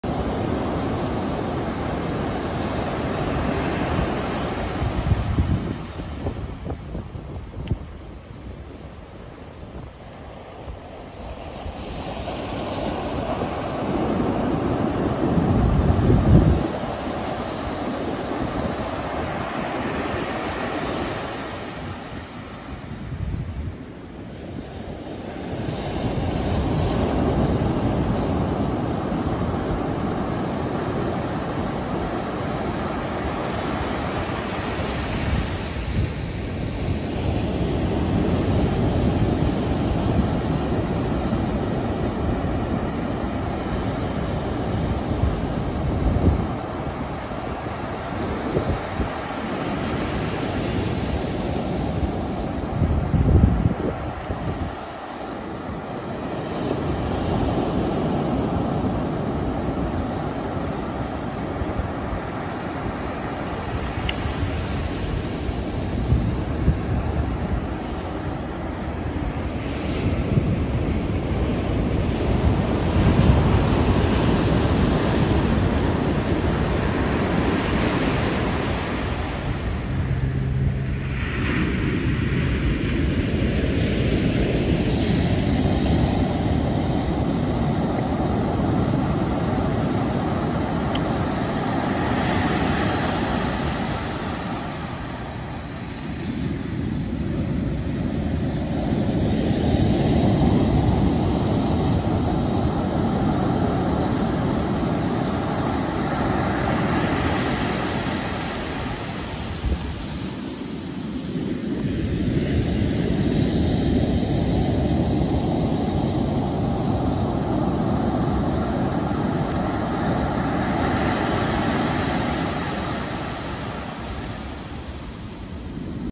Calming ocean waves in Laguna Beach
This is shot in winter with overcast near laguna beach.